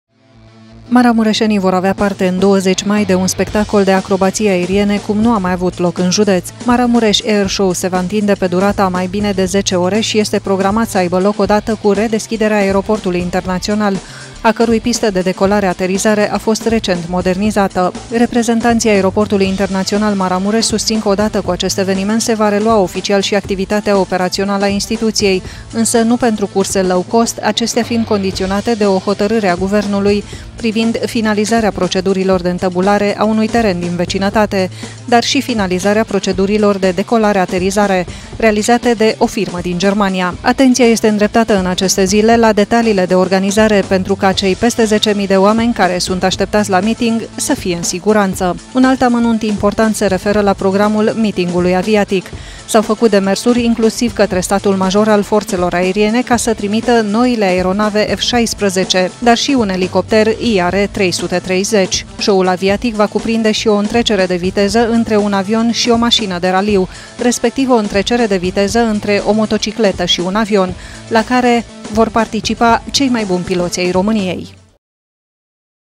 PREGĂTIRI PENTRU CEL MAI MARE MITING AVIATIC
Mobilizare intensa in aceste zile pentru pregatirea celui mai mare miting aviatic din judetul Maramures. Conducerea Aeroportului a facut demersuri inclusiv catre Statul Major al Fortelor Aeriene sa dispuna de deplasarea unor aeronave F16 la acest eveniment de amploare.